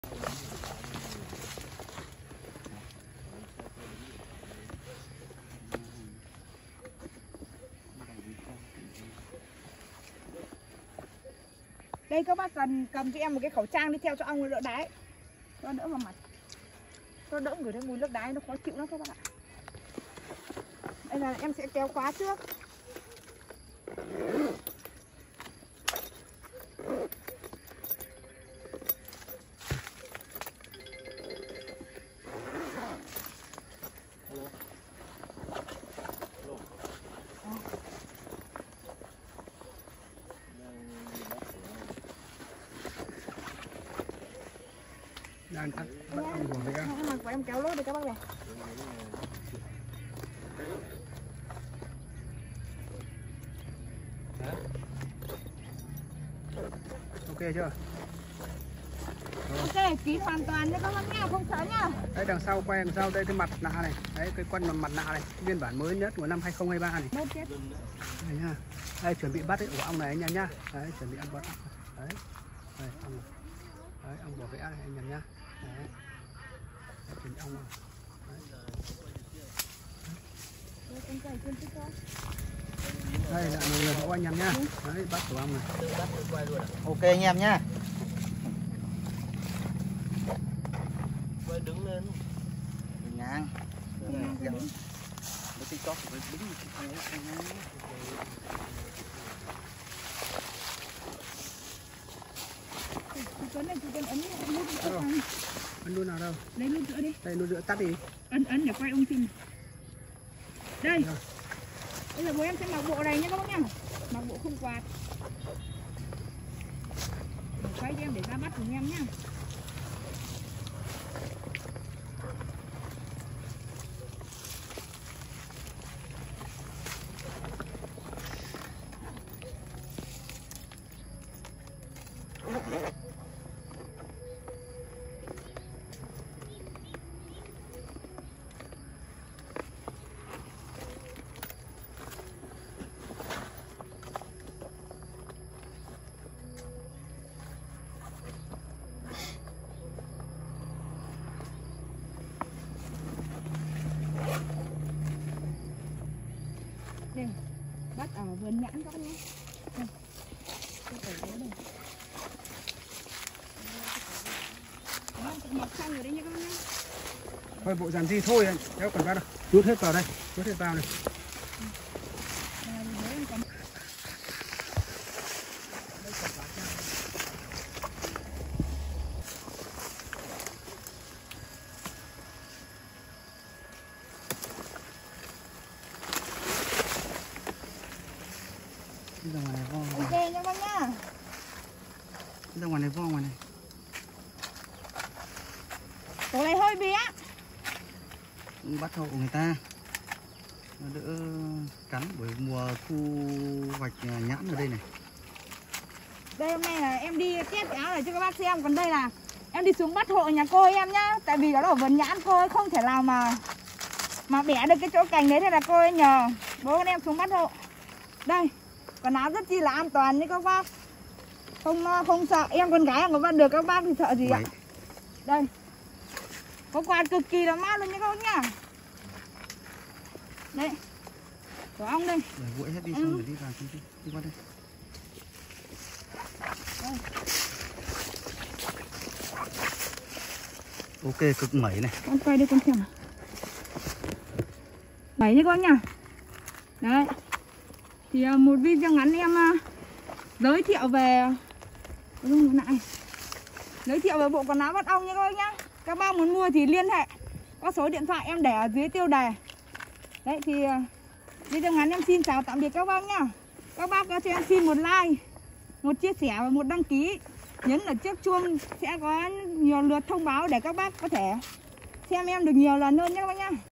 Con gái đi bắt ong vò vẽ và cái kết
Test thực tế bắt tổ ong đất Siêu To bằng quần áo bảo hộ bắt ong rừng hàng may thủ công TRung Quốc

Loa TOA bẫy chim đêm giá rẻ/ âm ly, loa phóng , loa kèn, lưới bẫy chim đêm giá 350k bao ship

🔰 Cảm ơn Tất Cả Mọi Người Đã Xem Clip Của Mình